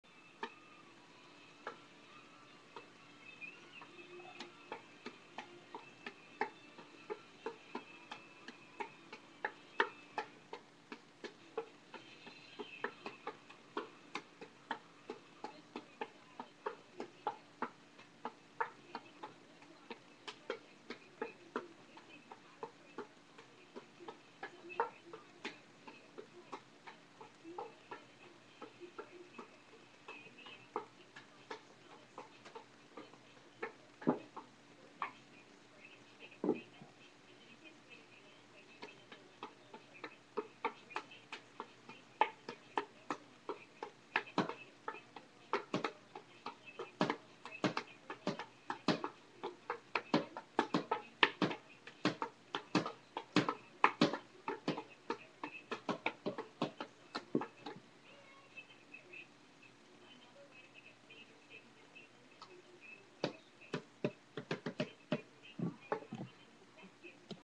me juggling clubs
just messing around not really trying much. but i can do alot better then this.